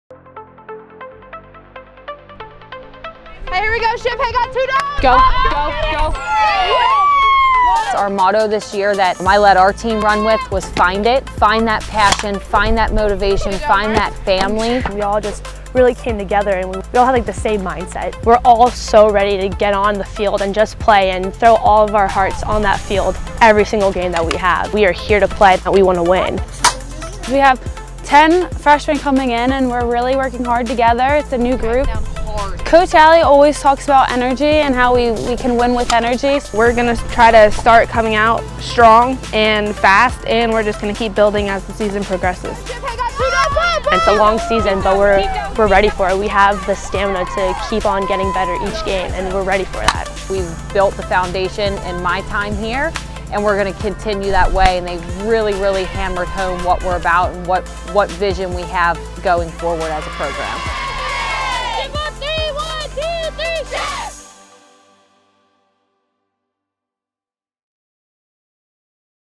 2019 VIDEO PREVIEW: Softball
VIDEO: Ship Softball opens its season this weekend with four games in North Carolina. SU has nine returning players and 12 newcomers (including 10 freshmen) looking to come together to help the Raiders reach the PSAC Tournament for the third straight season.